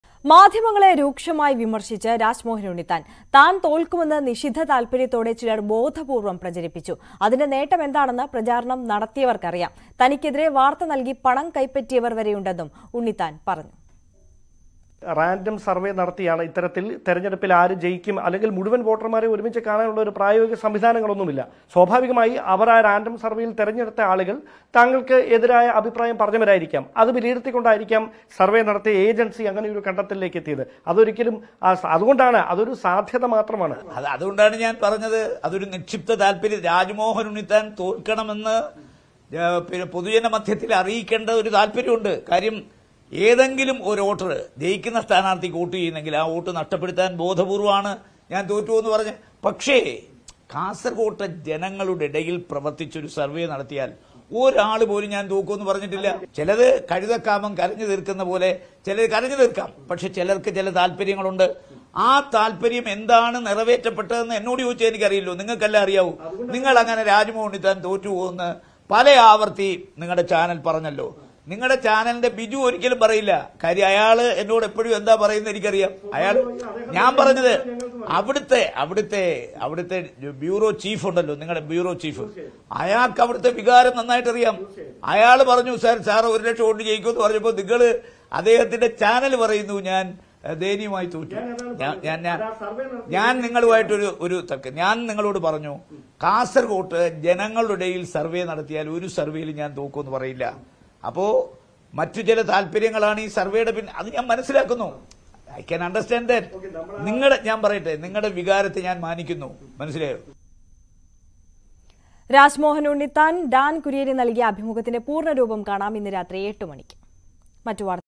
''Rajmohan Unnithan തോൽക്കുമെന്ന് പൊതുജനമധ്യത്തിൽ അറിയിക്കേണ്ടത് അവരുടെ താല്പര്യമായിരുന്നു''
Rajmohan Unnithan Exclusive Interview: CPMന്റെ അച്ചാരം പറ്റി തന്നെ പരാജയപ്പെടുത്താൻ Congressലുള്ള ചിലർ തന്നെ Kasargod തോൽപ്പിക്കാൻ ശ്രമിച്ചുവെന്ന് രാജ് മോഹൻ ഉണ്ണിത്താൻ. കൊലച്ചോർ ഉണ്ടവരോട് സൗഹൃദം പോലും പാടില്ല എന്ന നിലപാടാണ് Partyക്കുള്ളതെന്നും രാജ്  മോഹൻ ഉണ്ണിത്താൻ News18ന്  നൽകിയ പ്രത്യേക Interviewലായിരുന്നു  രാജ്‌മോഹൻ ഉണ്ണിത്താന്റെ വെളിപ്പെടുത്തൽ.